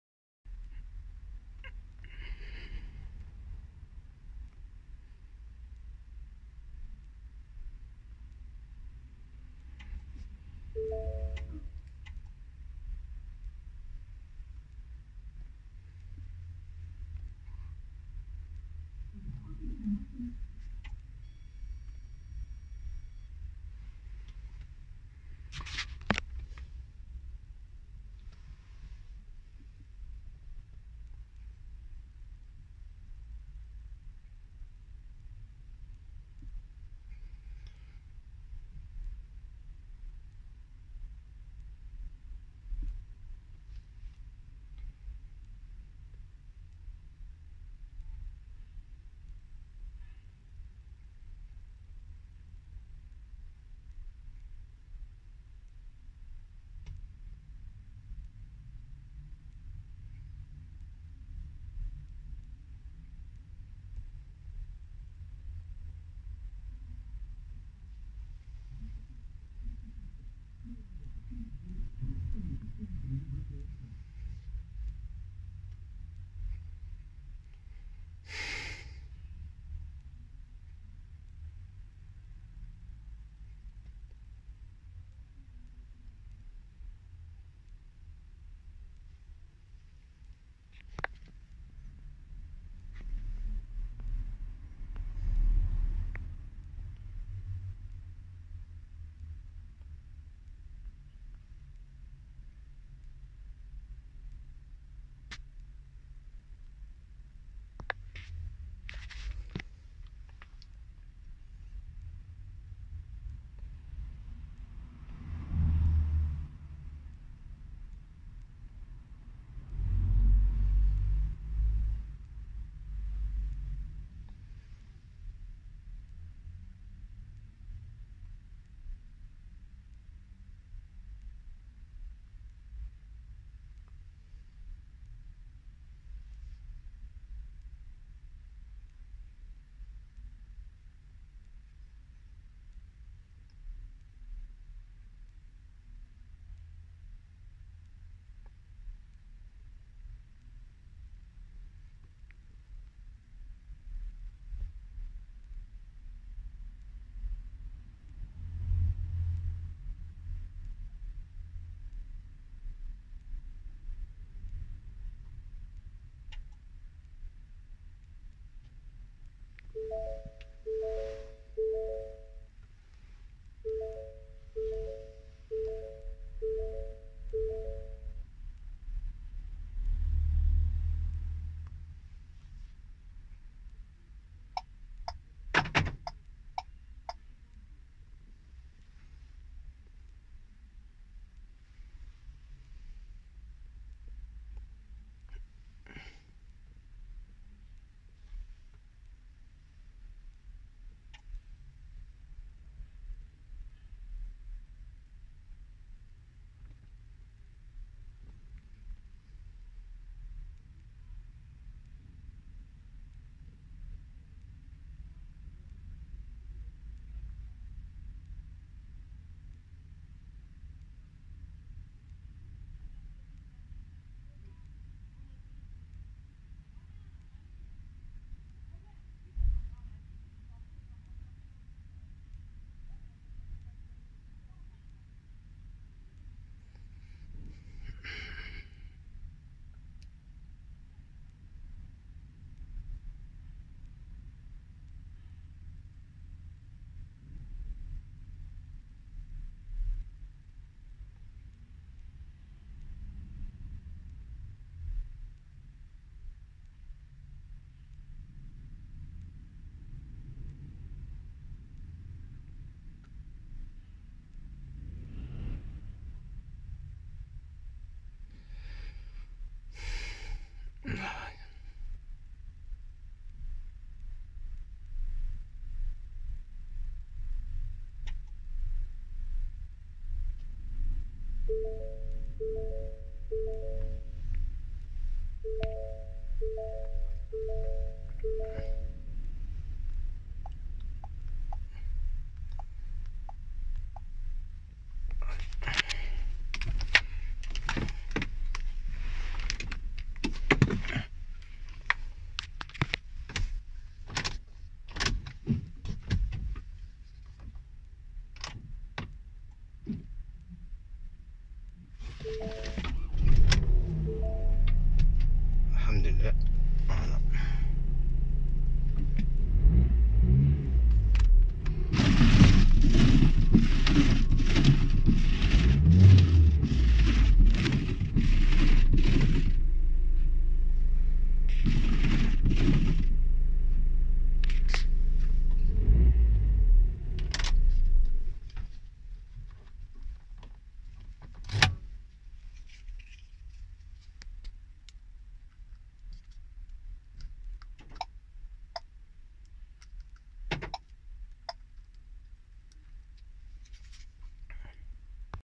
program key Ford transit 2020..reproduction clé Ford transit 2020
Prévoir un booster pour la batterie et une bonne connexion internet